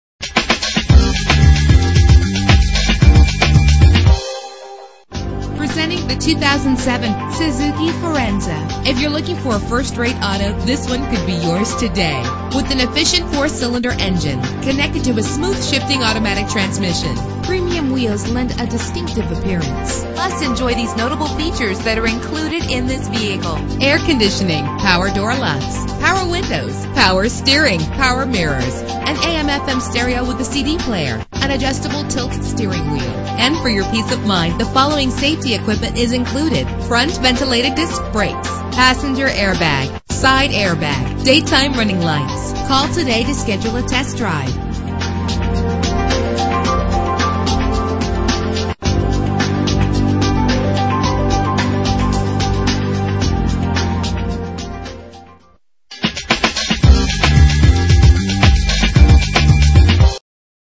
2007 Suzuki Forenza - Lynchburg VA
Year: 2007
Make: Suzuki
Model: Forenza
Trim: Base
Engine: 2.0 liter inline 4 cylinder 16 valve
Transmission: 4-Speed Automatic
Color: Fantasy Black Metallic
Mileage: 82962
Address:
19018 Forest Rd